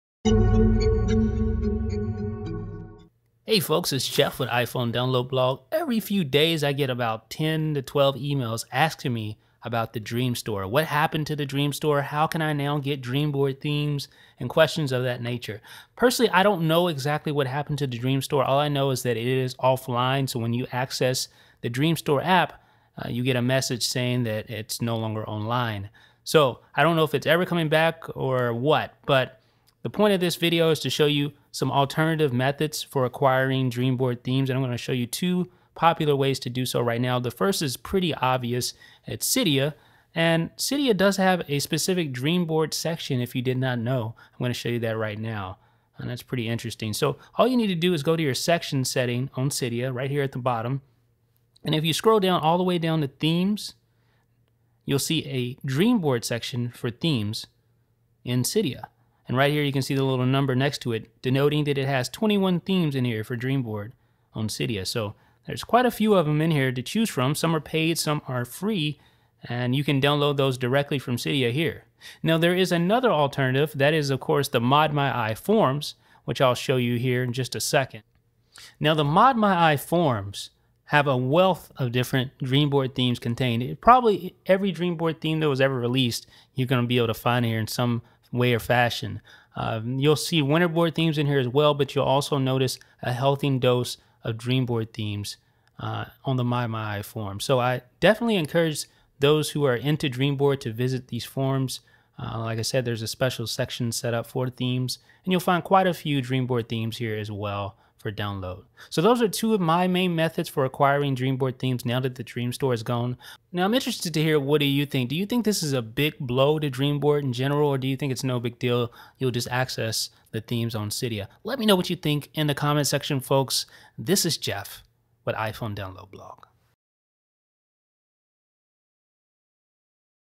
DreamStore Alternatives for DreamBoard Themes
Like this video? Share on Twitter!

Now that the DreamStore is no longer functioning, I'll discuss a couple of alternative ways to acquire those beautiful DreamBoard themes.

About iDB